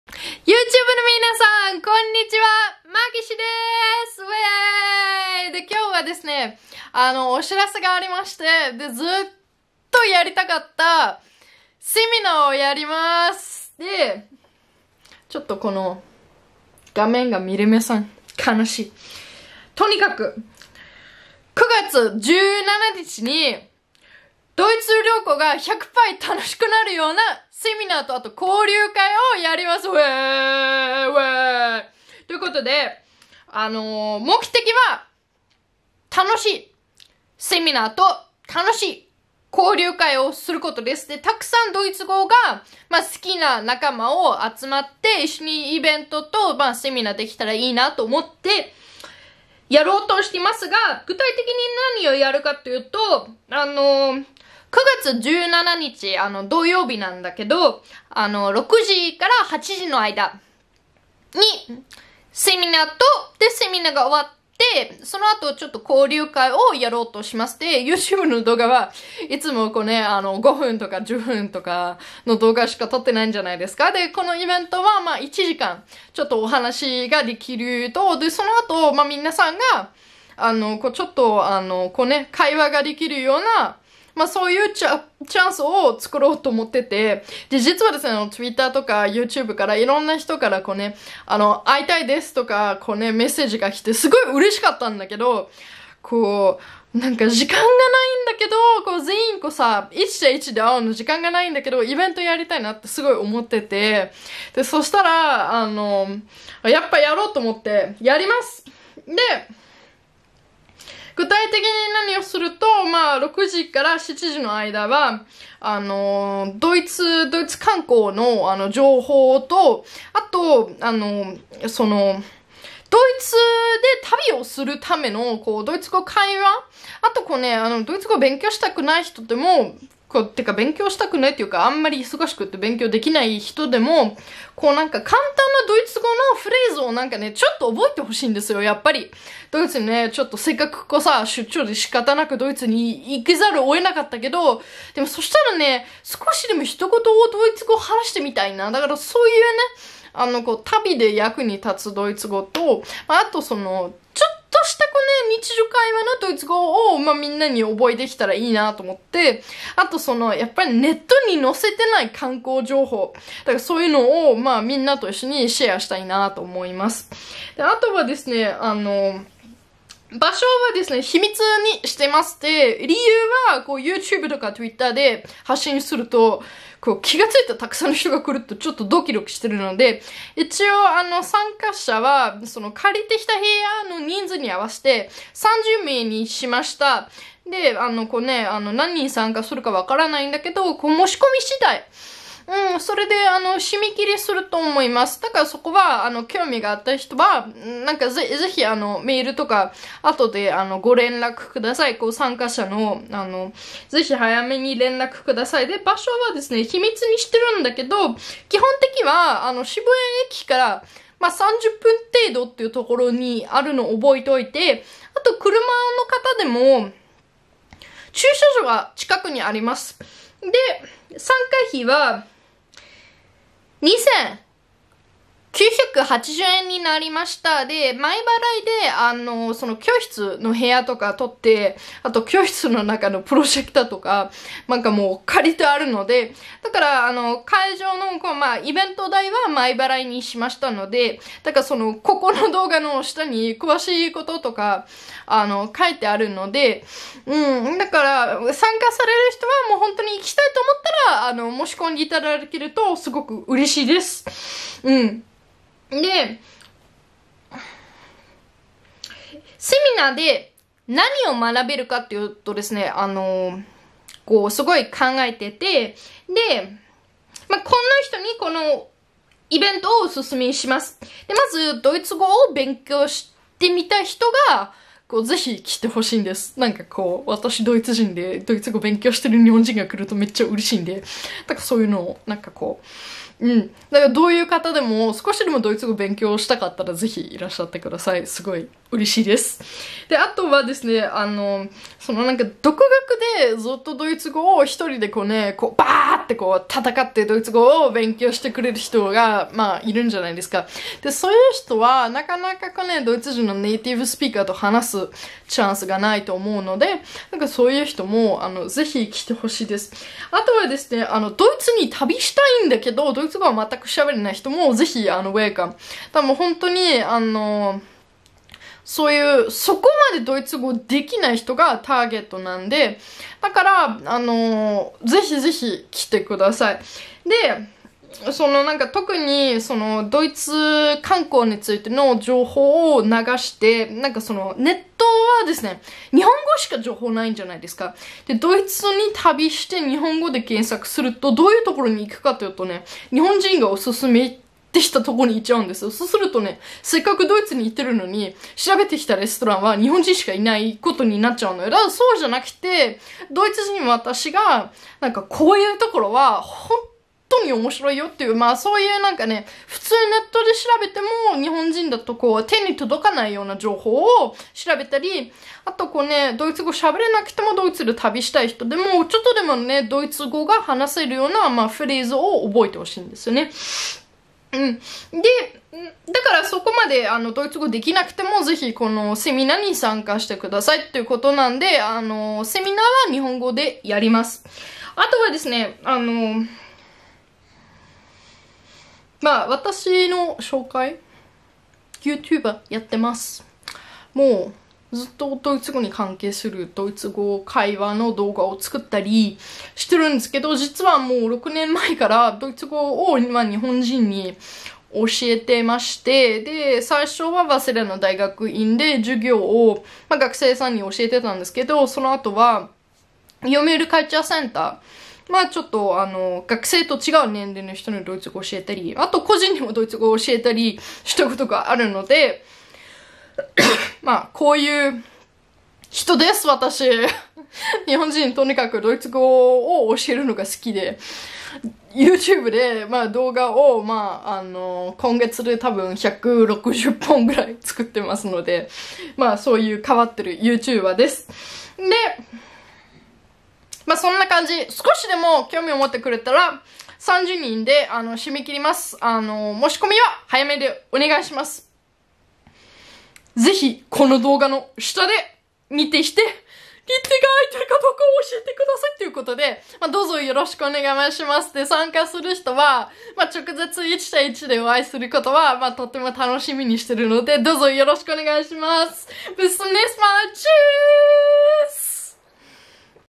YouTube Event
9月7日開催！
ドイツ語学習の効率がグッとあがる！・ドイツ旅行が100倍楽しくなる
セミナー（交流会）のお知らせ

セミナー（交流会）で学べること
このような方におすすめします！
・ドイツ語を勉強してみたい、興味があるが、何から手をつけていいかわからない！
・ドイツ語を学習しているが、効率のいい勉強方法を教えて欲しい。勉強仲間が欲しい！
・ネイティブスピーカーと話して実力を知りたい。ドイツ語を話してみたい！
・ドイツに旅行したい、留学したい。おススメの観光地・場所を知りたい！
・ネットや雑誌では紹介されていない、ドイツ人がおススメする人気スポットを知りたい！
日本語でのセミナーになります。
ドイツ語にを学んだことがない方、初学者、中級レベルの方を対象としています。

セミナー（交流会）概要
日時：　2019年9月7日（土）18：00～20：00
プログラム：
17：45～受付開始
〈第1部〉18：00～19：10　ドイツ語・ドイツ旅行に関する講座
〈第2部〉19：10～20：00　ドイツ語会話/グループレッスン（交流会）
講師：　ピカット マキシー
会場：　渋谷駅から30分で、教室の最寄駅です。
参加される方にはお申込み後、住所をお送りいたします。
料金：　2,980円（税込み）
定員：　30名（申し込み先着順）

講師紹介 Pickert Maxie（ピカット マキシー）
ボン大学、早稲田大学院卒業。14歳の時にドイツ語に訳された村上春樹の小説に出会い、日本に憧れる。16歳で、埼玉県の高校の1年間の交換留学プログラムを利用して初来日し、早稲田大学・大学院で日本語の勉強を継続。日本でドイツ語を勉強している人を手伝いたいという思いから、YouTubeチャンネルやTwitterを通じて定期的にドイツ・ドイツ語に関する情報発信している。

参加申し込みについて
1   クレジットカードもしくは、銀行振込での前払い制度をとらせていただいております。（教室費が発生するから前払いにしました !(_._)！）
2    マキシー宛てに以下メッセージをください。

・氏名（必須）
・セミナーに期待すること、学びたいこと（任意、あればで結構です。）

定員つきましては、先着順でのお受付とさせていただいております。お早めにお申込みください。

振込先
クレジットカードお支払い

銀行振込
メッセージください。

Facebook Maxie Pickert
LinkedIn Maxie Pickert